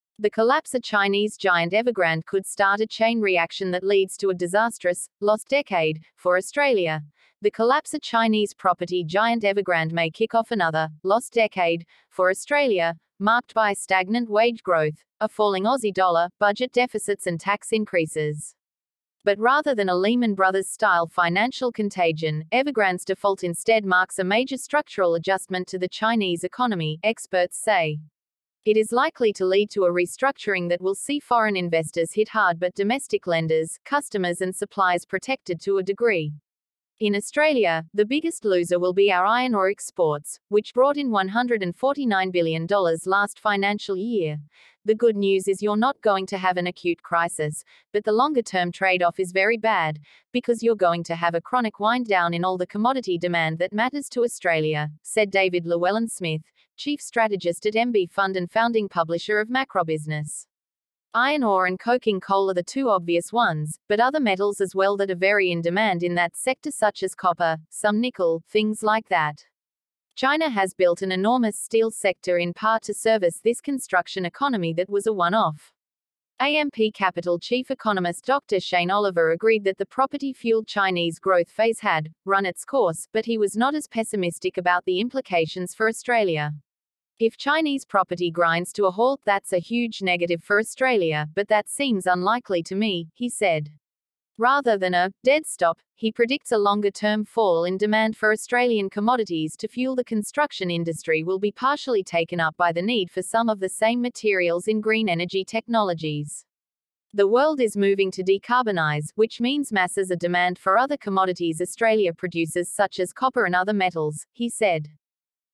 'Another lost decade': What Evergrande's collapse means for Australia
The collapse of Chinese giant Evergrande could start a chain reaction that leads to a disastrous “lost decade” for Australia.The collapse of Chinese property giant Evergrande may kick off another “lost decade” for Australia, marked by stagnant wage growth, a falling Aussie dollar, budget deficits and tax increases.
But rather than a Lehman Brothers-style financial contagion, Evergrande’s default instead marks a major structural adjustment to the Chinese economy, experts say. It is likely to lead to a restructuring that will see foreign investors hit hard but domestic lenders, customers and suppliers protected to a degree.
In Australia, the biggest loser will be our iron ore exports, which brought in $149 billion last financial year.“The good news is you’re not going to have an acute crisis, but the longer-term trade-off is very bad, because you’re going to have a chronic wind-down in all the commodity demand that matters to Australia,” said David Llewellyn-Smith, chief strategist at MB Fund and founding publisher of MacroBusiness.

“Iron ore and coking coal are the two obvious ones, but other metals as well that are very in demand in that sector such as copper, some nickel, things like that. China has built an enormous steel sector in part to service this construction economy that was a one-off.”

AMP Capital chief economist Dr Shane Oliver agreed that the property-fuelled Chinese growth phase had “run its course”, but he was not as pessimistic about the implications for Australia.
“If Chinese property grinds to a halt that’s a huge negative for Australia, but that seems unlikely to me,” he said.
Rather than a “dead stop”, he predicts a longer-term fall in demand for Australian commodities to fuel the construction industry will be partially taken up by the need for some of the same materials in green energy technologies.
“The world is moving to decarbonise, which means masses of demand for other commodities Australia produces such as copper and other metals,” he said.